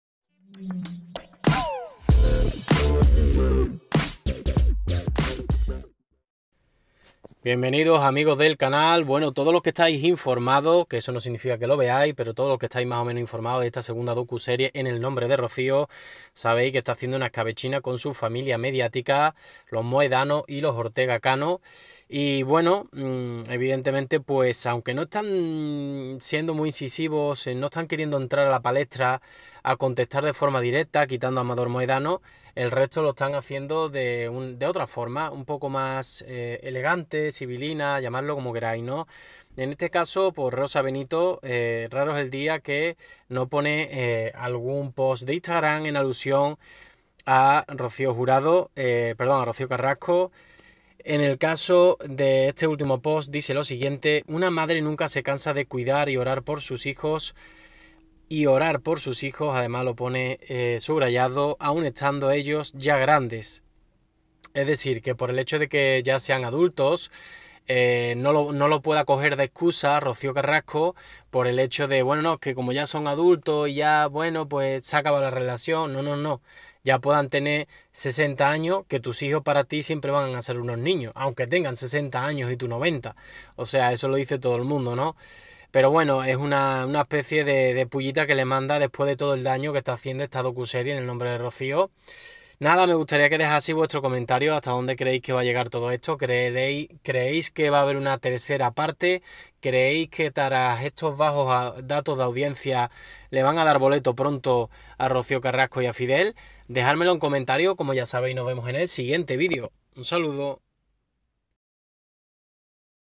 BRUTAL PULLA de Rosa Benito a Rocío Carrasco, por su DOCUSERIE en el nombre de Rocío
Cosméticos, hogar...